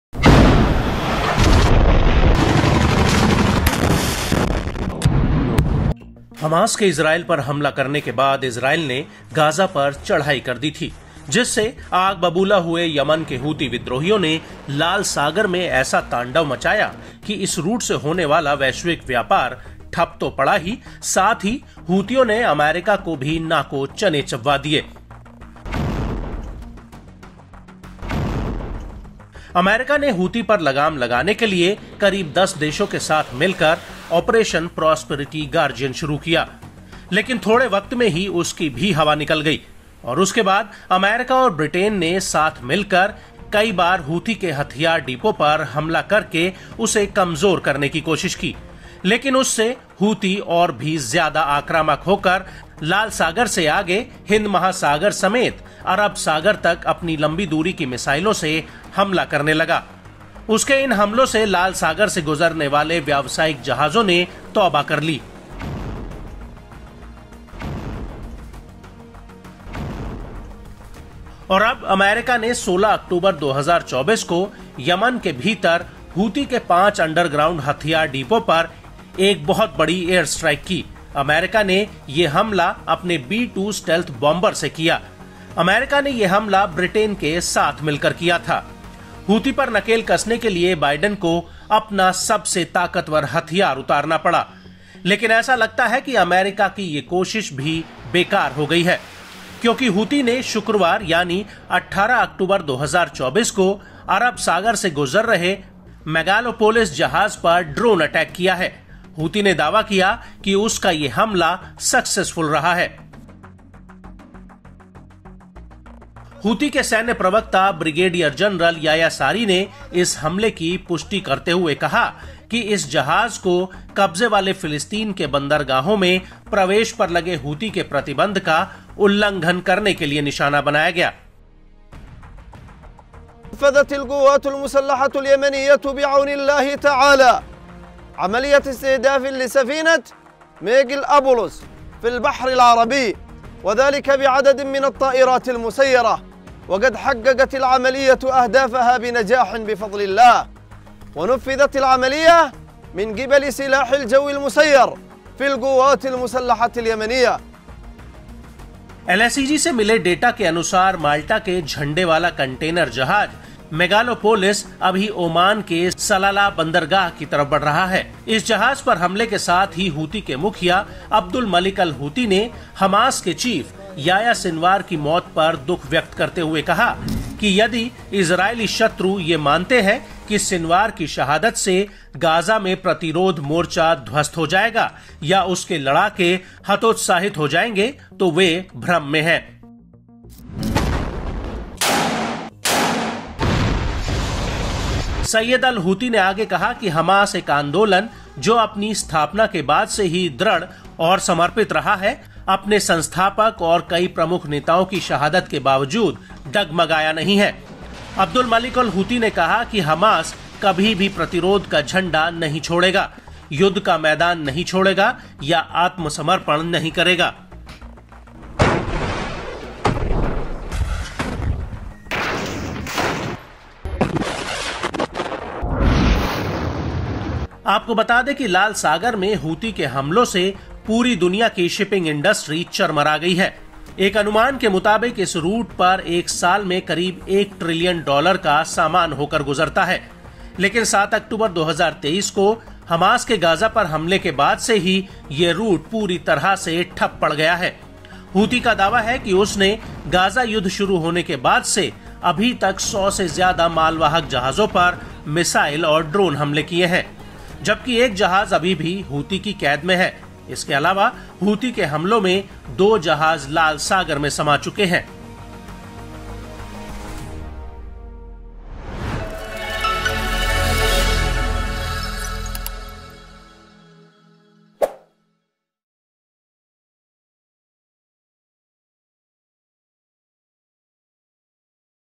Israel Hamas War : हूती ने तबाह किया अमेरिका का सबसे ताकतवर हथियार | Houthi drone Attack | N18G
America, along with about 10 countries, started Operation Prosperity Guardian to control Houthi, but it also failed in a short time. And after that, America and Britain together tried to weaken the Houthis by attacking their weapons depots many times, but due to this, the Houthis became even more aggressive and started attacking with their long-range missiles from the Red Sea to the Indian Ocean as well as the Arabian Sea. On October 16, 2024, America carried out a huge airstrike on 5 underground weapons depots of Houthi inside Yemen.. America carried out this attack with its B-2 Stealth Bombers.. America carried out this attack in collaboration with Britain.. To crack down on Houthi, Biden had to use his most powerful weapon, but it seems that this effort of America has also gone in vain. Because Houthi carried out a drone attack on the MEGALOPOLIS ship passing through the Arabian Sea on Friday i.e. 18 October 2024, Houthi claimed that its attack was successful.

अमेरिका ने हूती पर लगाम लगाने के लिए करीब 10 देशों के साथ मिलकर ऑपरेशन प्रोस्पेरिटी गार्डियन शुरू किया लेकिन थोड़े वक़्त में उसकी भी हवा निकल गई। और उसके बाद अमेरिका और ब्रिटेन ने साथ मिलकर कई बार हूती के हथियार डिपो पर हमला कर उसे कमज़ोर करने कि कोशिश की लेकिन उससे हूती और भी ज्यादा आक्रामक होकर लाल सागर से आगे हिन्द महासागर समेत अरब सागर तक अपनी लम्बी दूरी की मिसाइलों से हमले करने लगा. अमेरिका ने 16 अक्टूबर 2024 को यमन के भीतर हूती के 5 अंडरग्राउंड हथियार डिपो पर एक बहुत बड़ी एयरस्ट्राइक की.. अमेरिका ने यह हमला अपने B-2 Stealth Bombers से किया... अमेरिका ने ये हमला ब्रिटेन के साथ मिलकर किया था.. हूती पर नकेल कसने के लिए बाइडेन को अपना सबसे ताकतवर हथियार उतारने पड़ा लेकिन ऐसा लगता है की अमेरिका की ये कोशिश भी बेकार हो गई है, क्योंकि हूती ने शुक्रवार यानि 18 अक्टूबर 2024 को अरब सागर से गुज़र रहे MEGALOPOLIS जहाज़ पर ड्रोन अटैक किया, हूती ने दवा किया की उसका ये हमला successful रहा